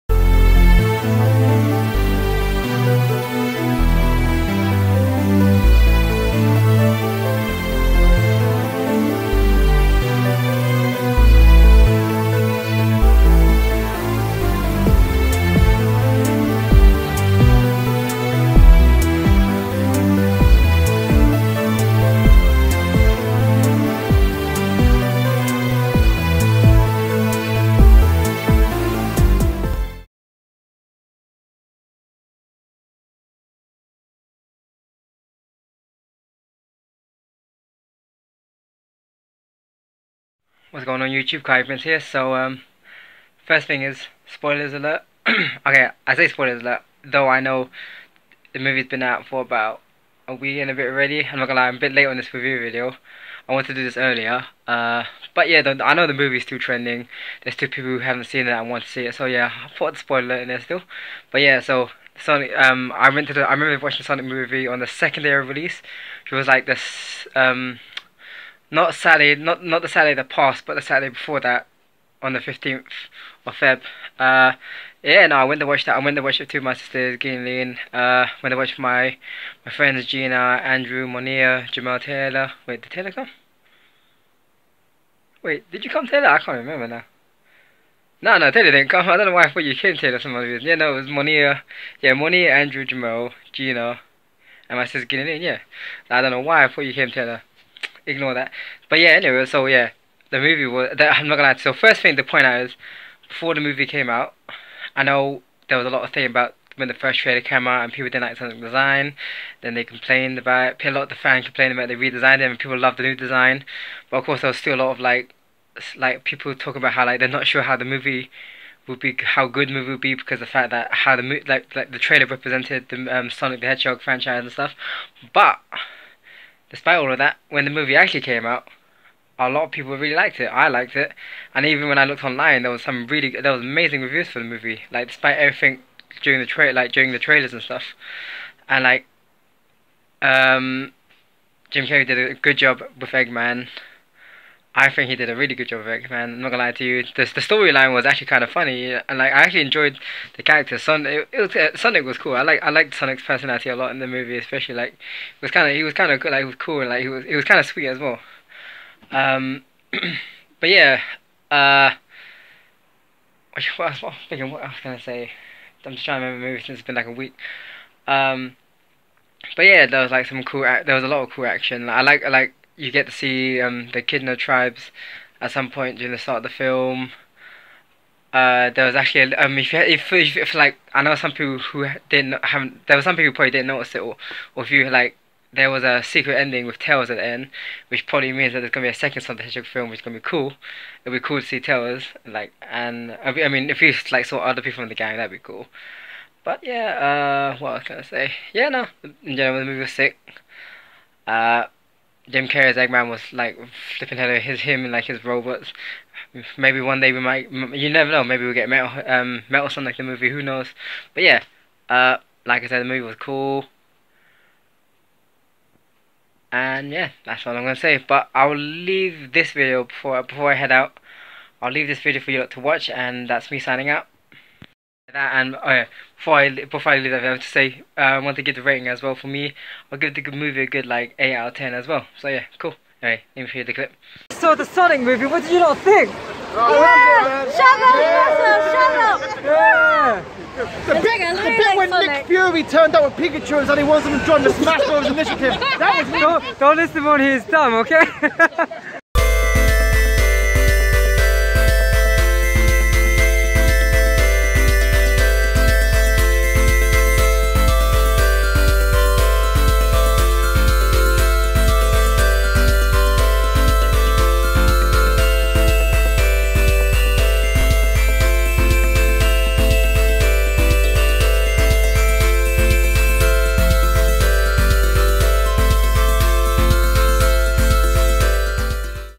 (Late Upload Series) Sonic the Hedgehog Movie Review
(Late Upload Series)
A series of videos I was meant to edit and upload during an earlier date but didn't, but still wanted to show you all.

This video I was meant to edit and upload earlier was of me reviewing the Sonic the Hedgehog movie. I had watched the movie with my friends and sisters months ago at the cinemas on 15th February.

Intro and outro music produced by me:
-Intro song

A Shout out to my friends and sisters in this video:
-Gina (Hedgey Art)
Thanks to her for her ending avatar work.

Lien
Andrew